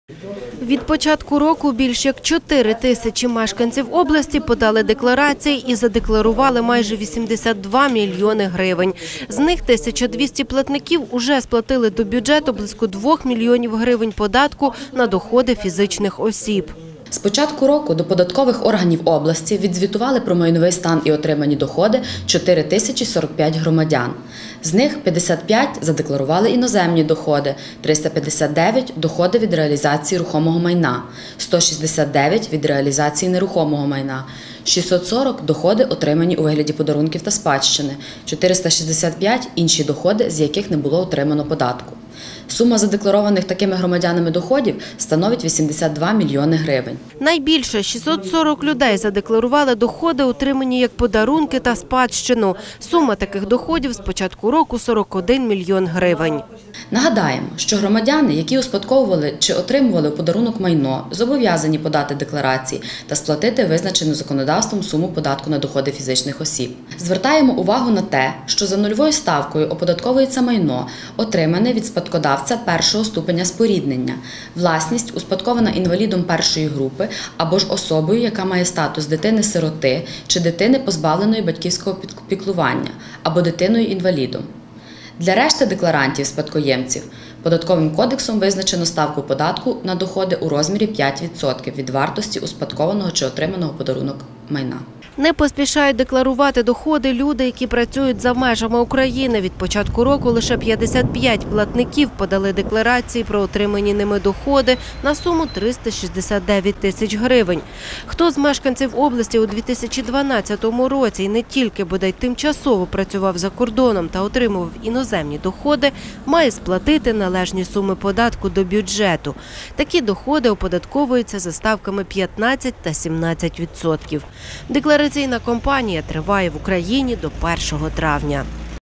Триває деклараційна кампанія
З початку року в Україні триває деклараційна кампанія. Станом на кінець березня до податкових органів області відзвітували про майновий стан та отримані доходи більш як 4 тисячі громадян. Найбільше цьогоріч, розповідають у Державній податковій службі, декларують доходи, отримані у вигляді подарунків та спадщини. Сума таких декларацій майже 28 мільйонів гривень.